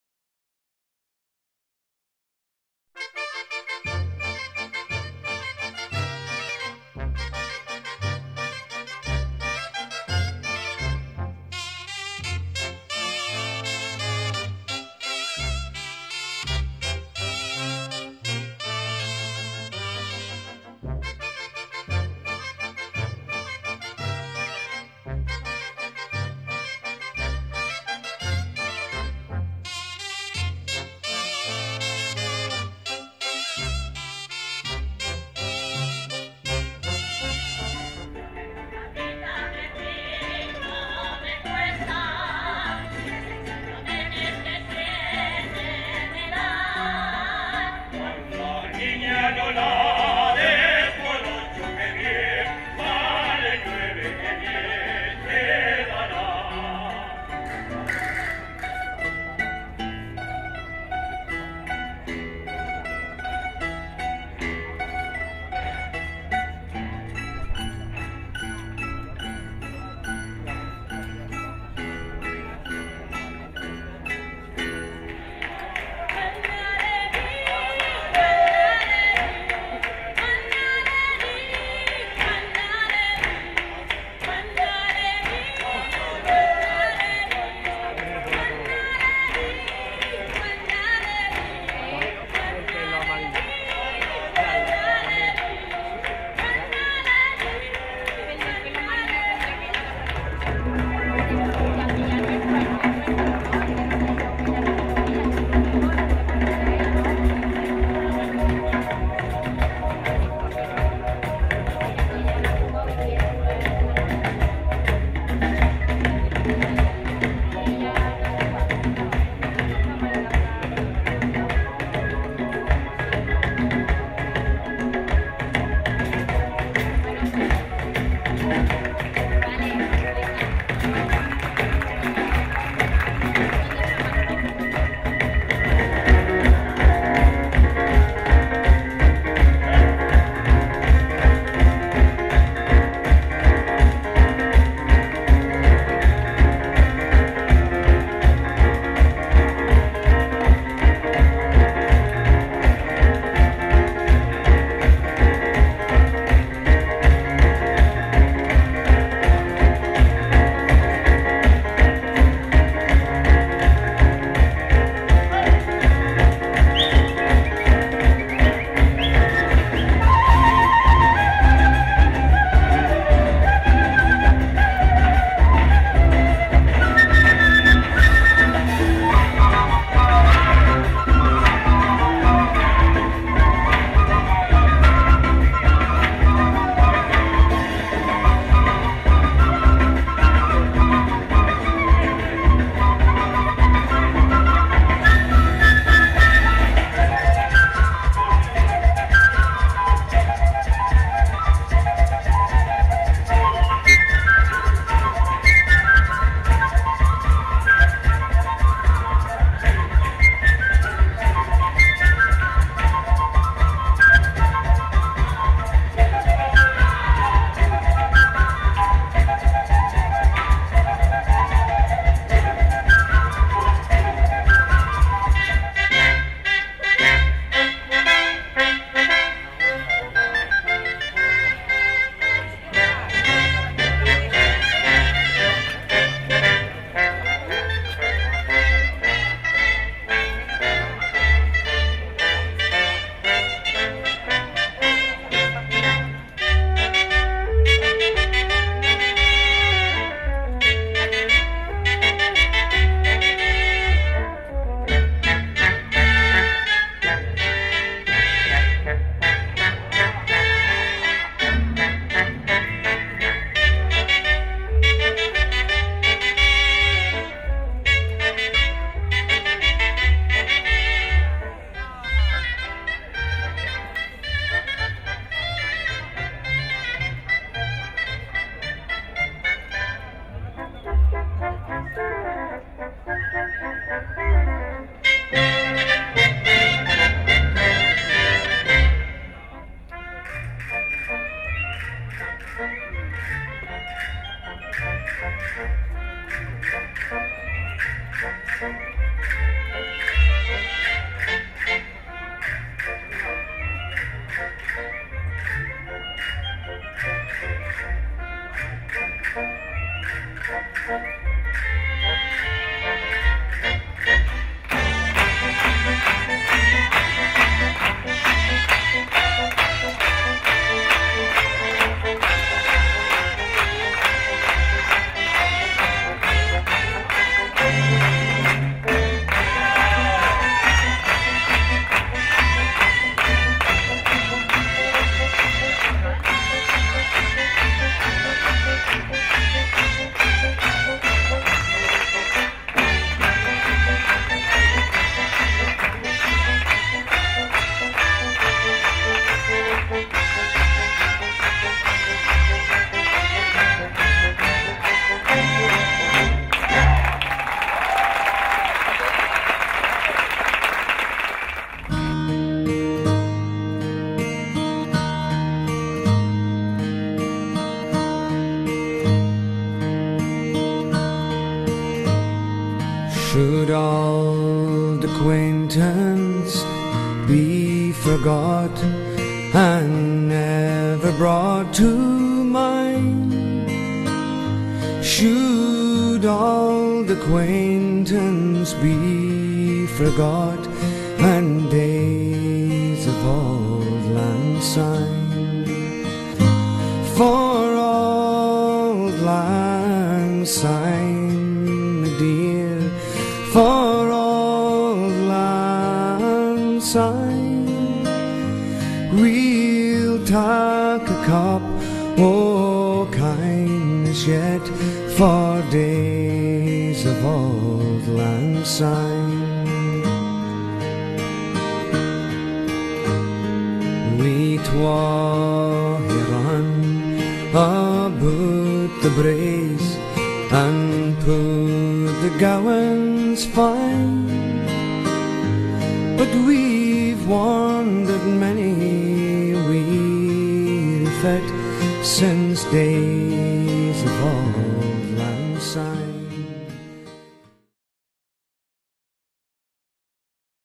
Rubifolk 2013
Espectacle de cloenda de l´edició del Rubifolk 2013 a l´Auditori del Castell de Rubí, diumenge 7 de juliol a les 19:30h